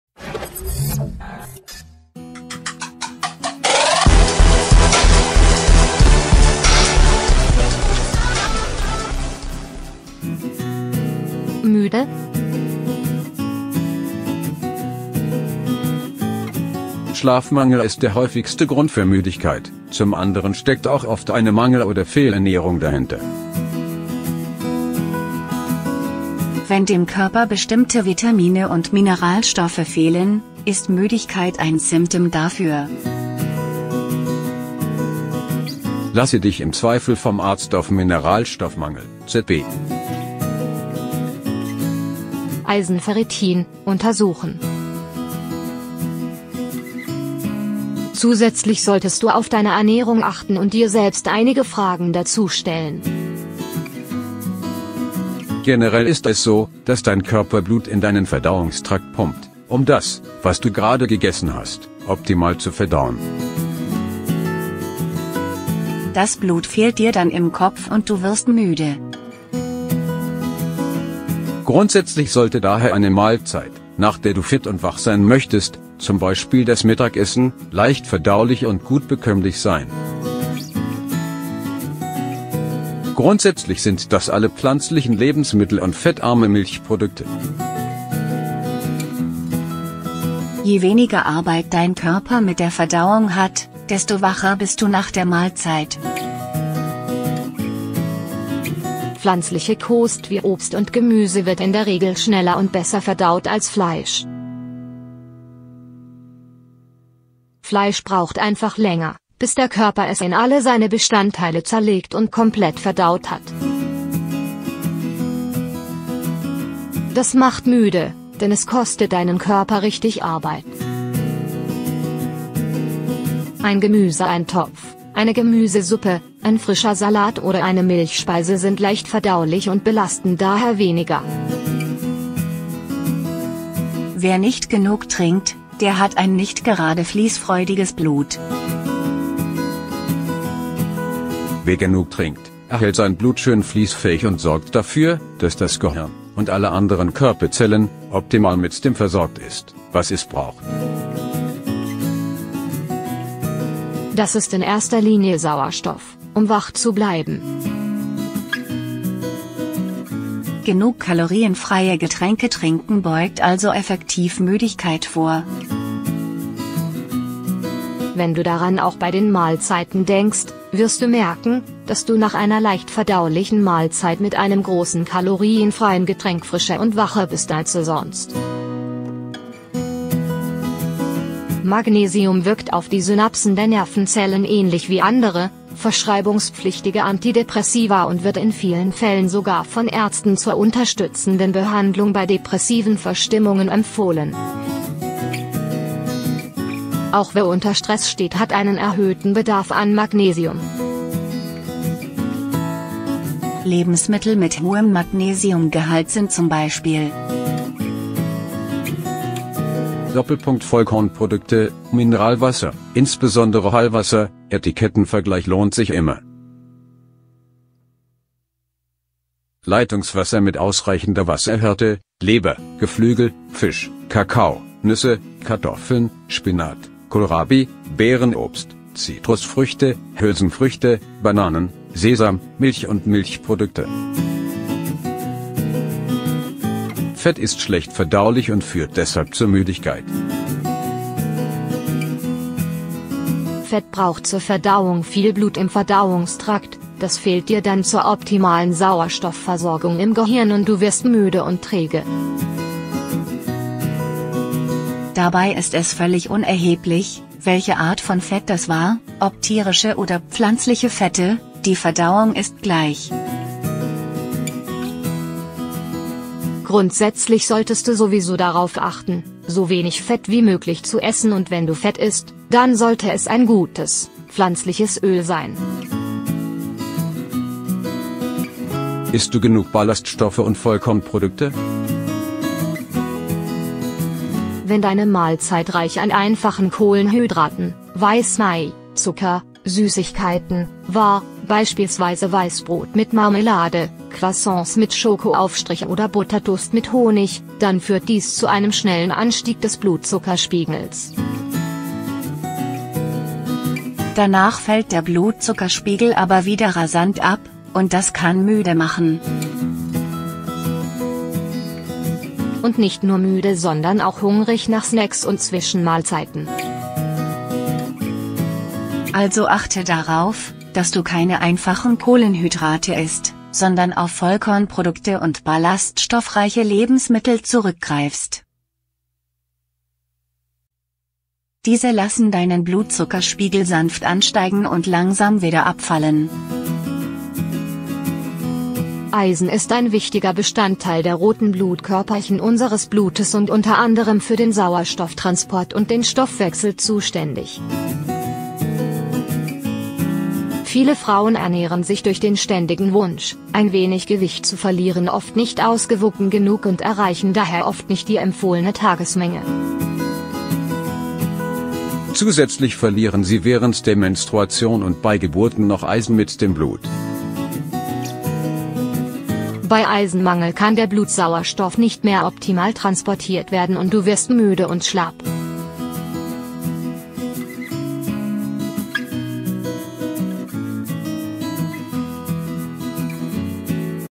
Du bist oft müde? Vielleicht ernährst du dich falsch!
Wenn du oft müde bist, kann auch eine Fehlernährung der Grund sein